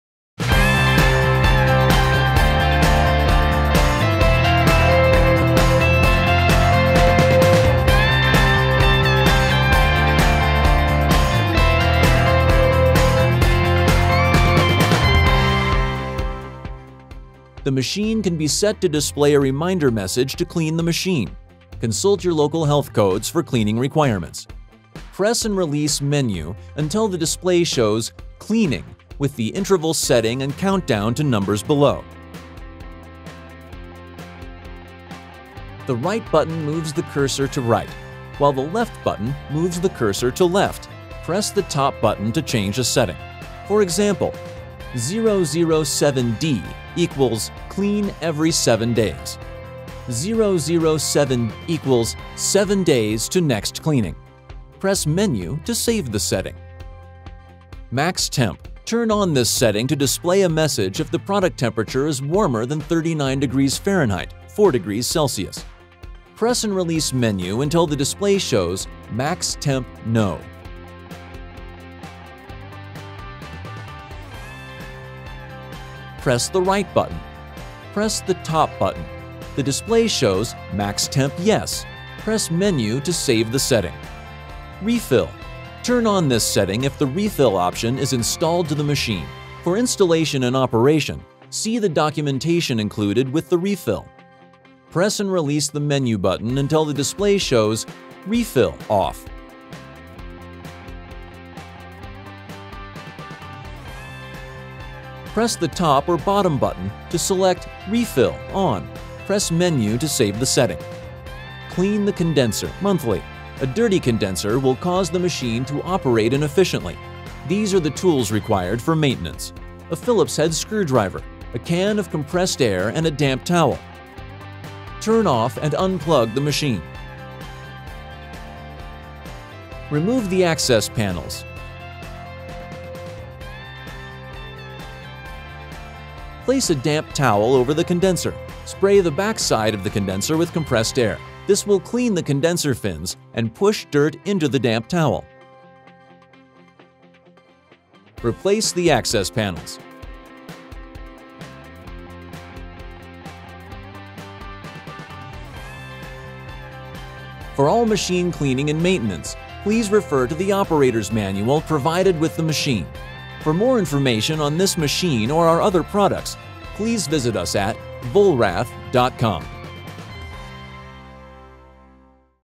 Vollrath Frozen Beverage Granita Machine - Advanced Settings & Maintenance
In this video, learn how to set advanced settings on your Vollrath Frozen Beverage Granita Machine. Settings shown include cleaning reminder, max temp, and refill. This video will also walk you through proper maintenance of your machine.